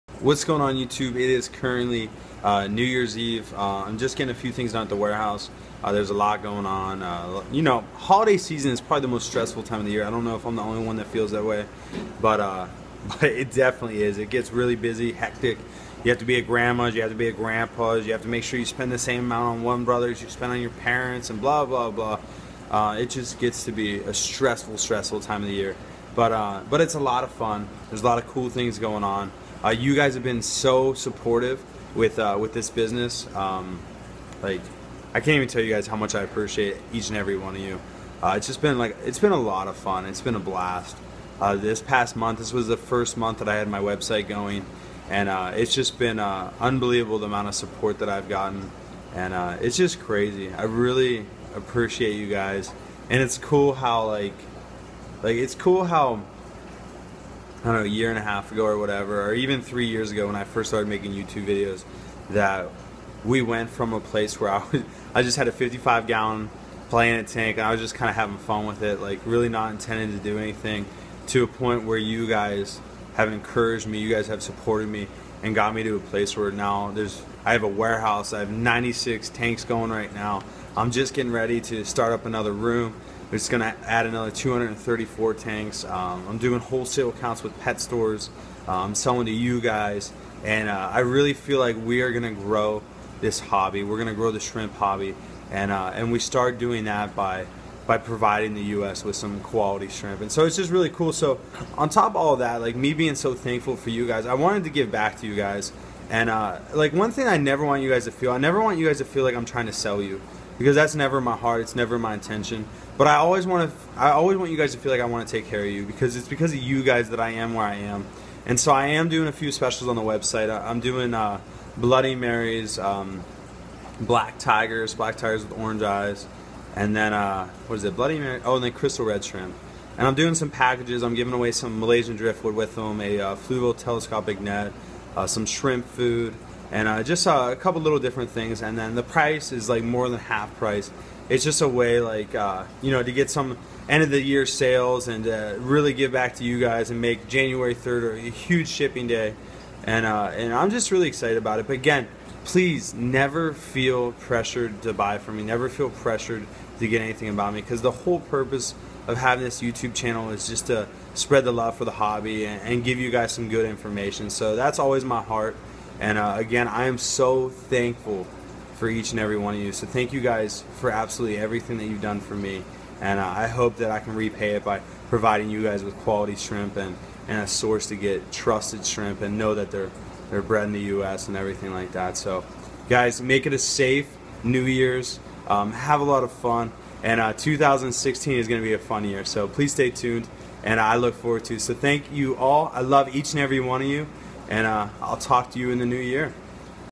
2015 Send off and a New Year Special
I am so incredibly blessed by each and every one of you. I want to thank you all for everything you have done for me: the support, encouragement, love, and everything between.

I wanted to run a special for the new year for you all of you. I know that when winter hits, all I want is to set up more tanks and get new species of shrimp. So, I knew I wouldn't be alone. Check out the special here:

I can not wait for 2016. The future of my business and the future of this hobby. Together, we are going to grow this hobby and I can not wait to see what this new year brings.

If you are looking for other shrimp or products. Feel free to buy freshwater shrimp here: